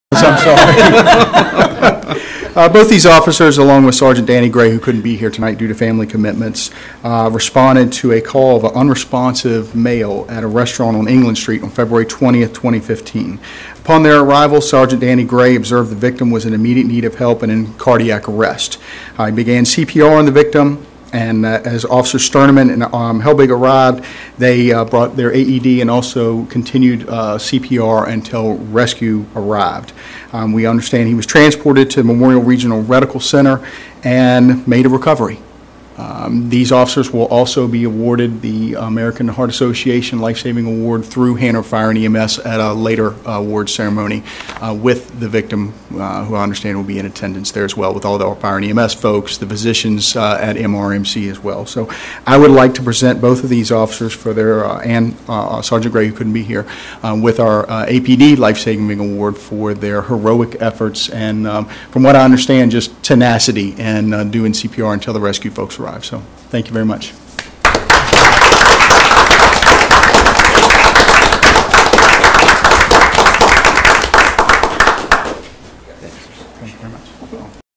APD Lifesaving Award
Chief Goodman awards Officer Stirnaman, Officer Helbig and Sergeant Gray with the APD Lifesaving Award.